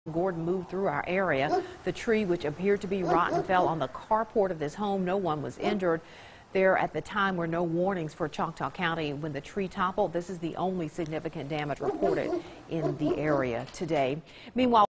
Tree on House 09-05-18
Remnants of tropical storm Gordon moved through north Mississippi.